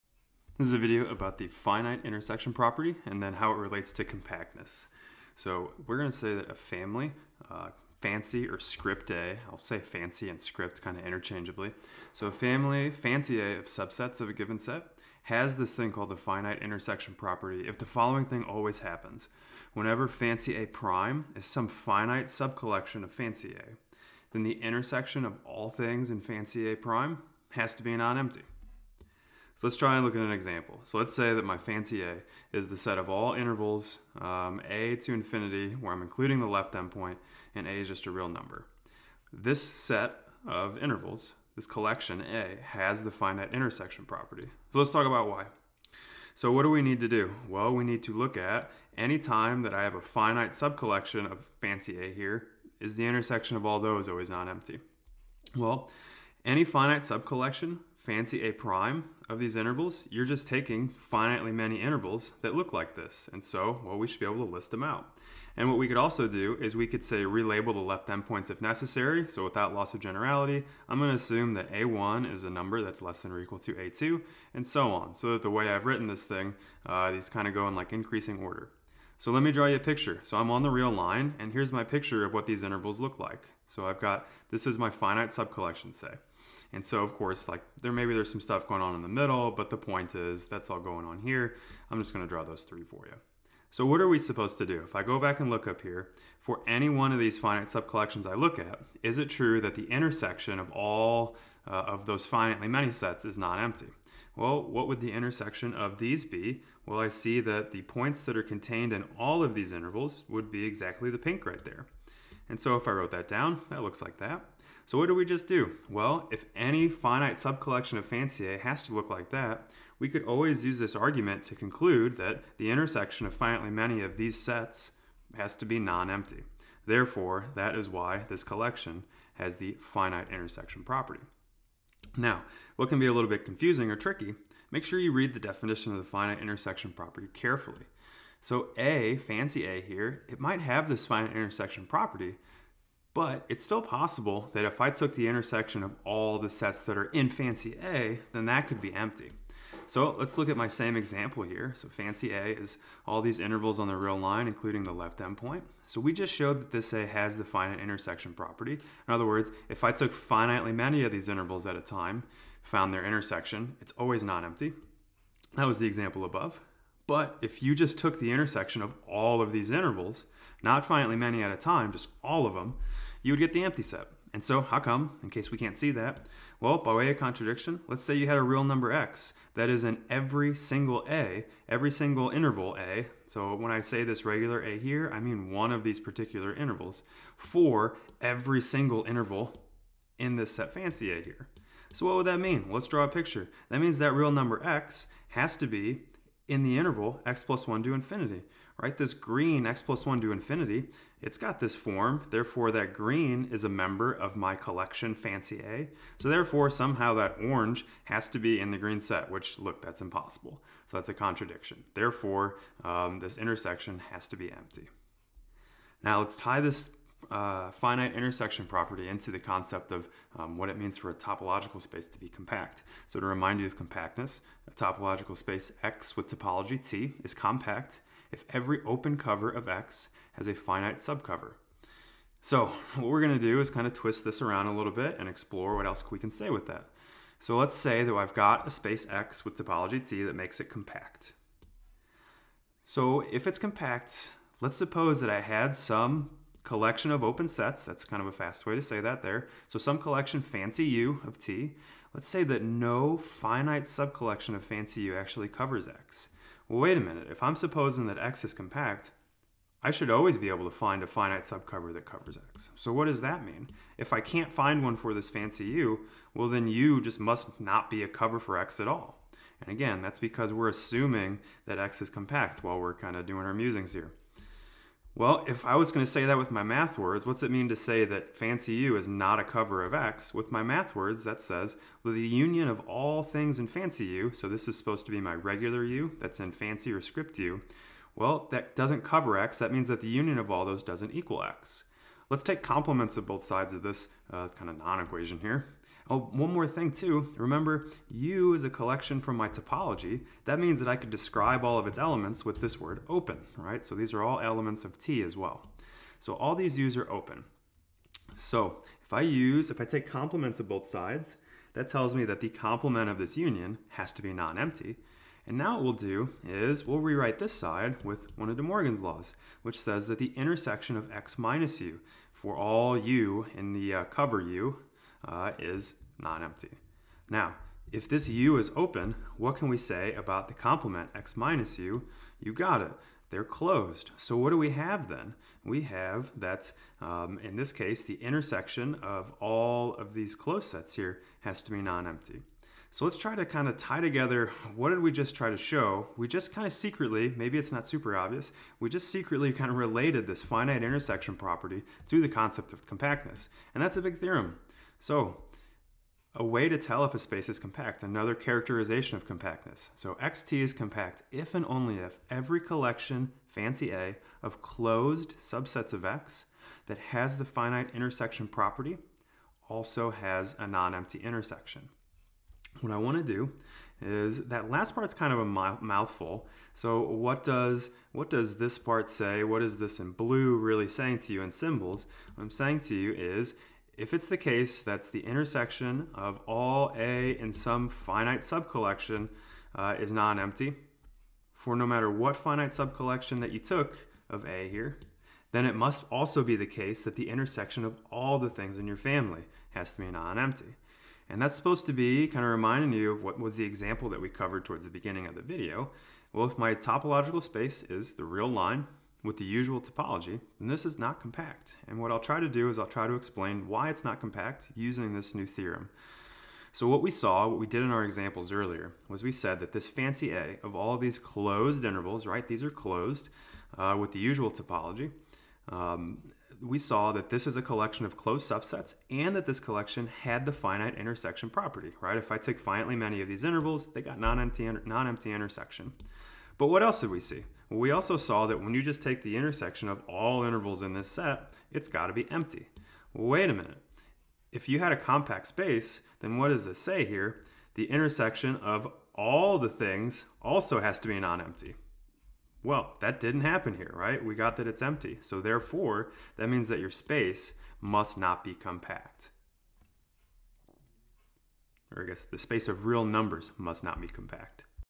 Finite intersection property and compactness
This is a short lecture about the finite intersection property, and how it relates to compactness in topological spaces. This is for my online topology class.